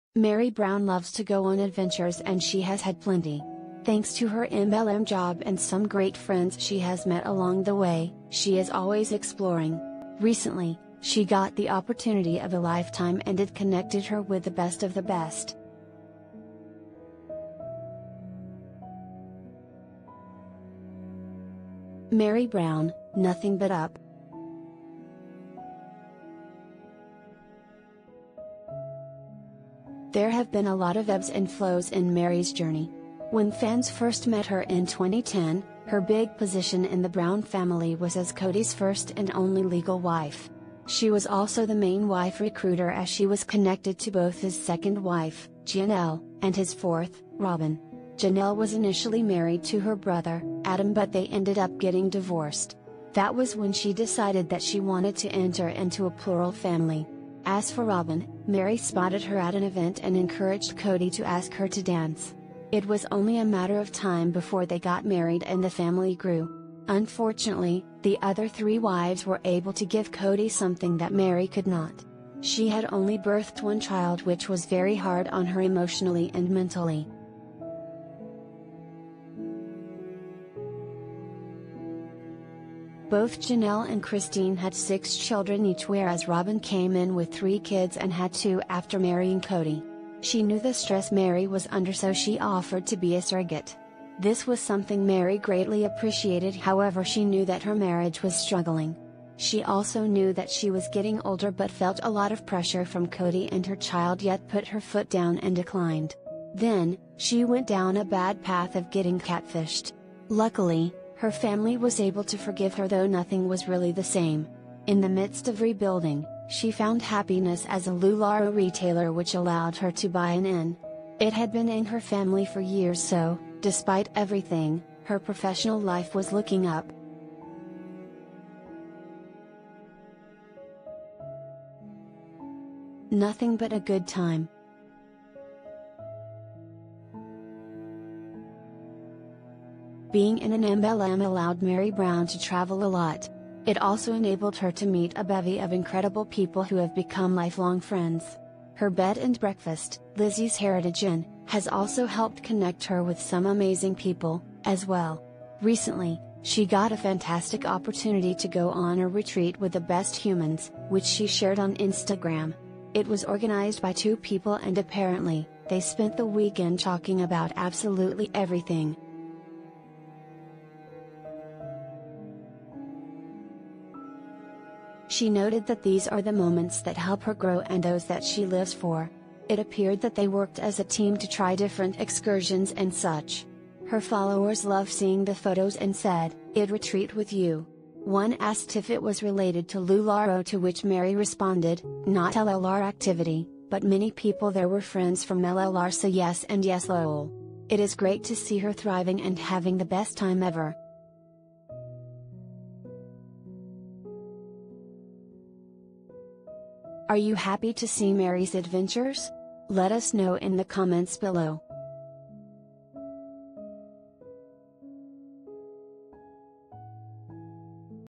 Meri Brown Goes On Exciting Trip With The 'Best Humans'
Meri Brown loves to go on adventures and she has had plenty. Thanks to her MLM job and some great friends she has met along the way, she is always exploring. Recently, she got the opportunity of a lifetime and it connected her with the best of the best.
Meri Brown, Nothing But Up
There have been a lot of ebbs and flows in Meri’s journey. When fans first met her in 2010, her big position in the Brown family was as Kody’s first and only legal wife. She was also the main wife recruiter as she w